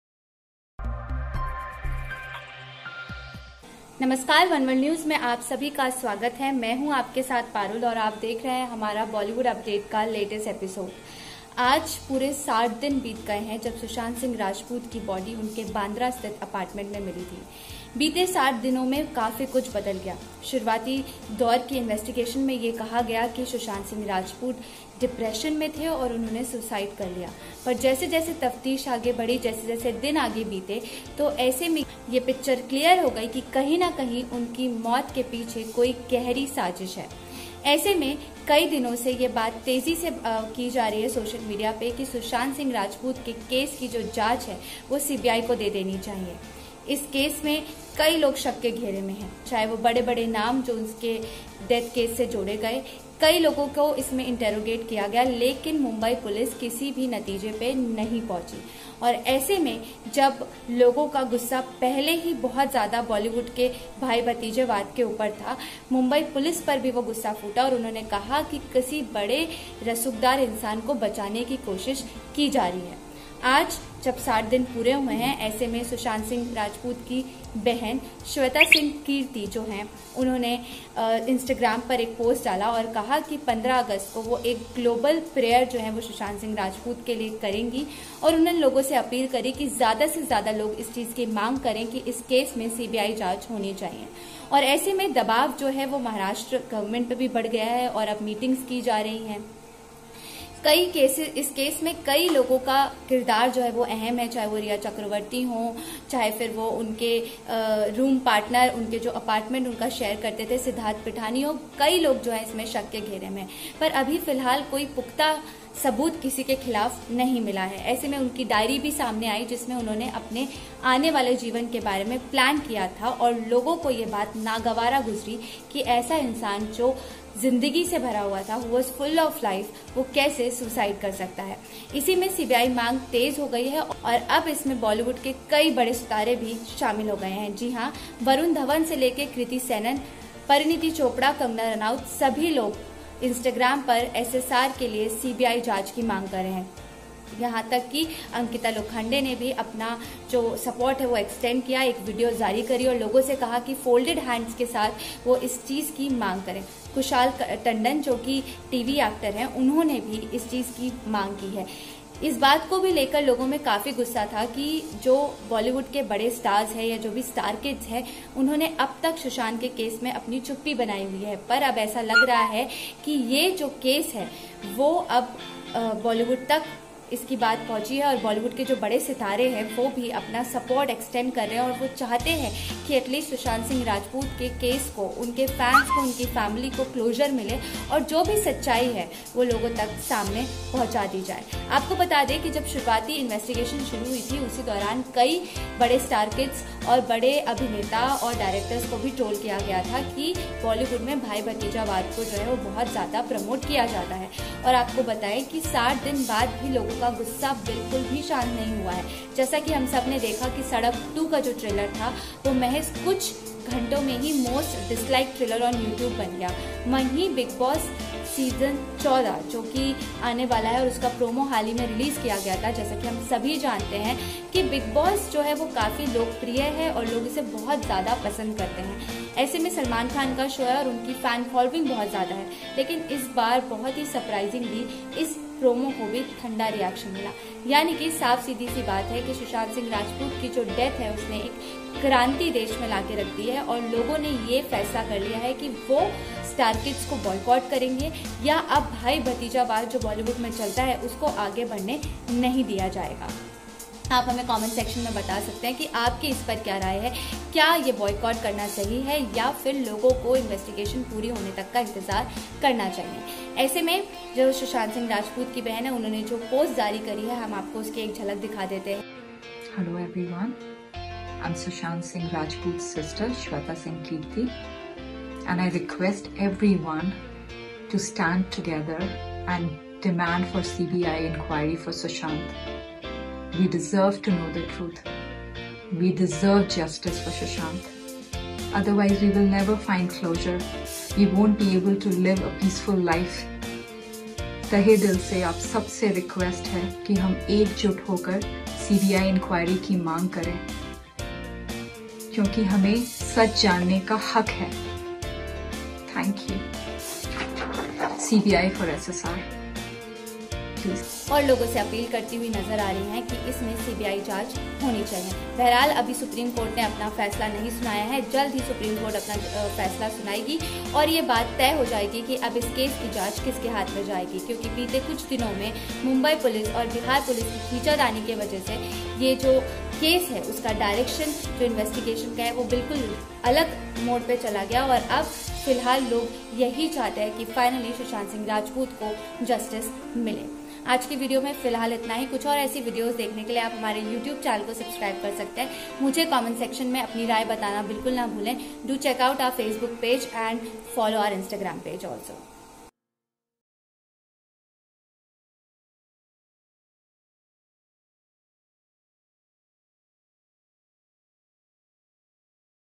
60 days of SSR' demise: 60 दिन बाद मे लोगों का गुस्सा नहीं हुआ शांत, आलिया और सलमान पर भड़के
बीते 60 दिनों मे बहुत कुछ बदल गया अगर कुछ नहीं बदला तो वो हैं सुशांत के फैंस का साहस, सीबीआई जांच को मिला  बॉलीवुड का साथ. अंकिता से लेकर वरुण धवन ने भी लगाई गुहार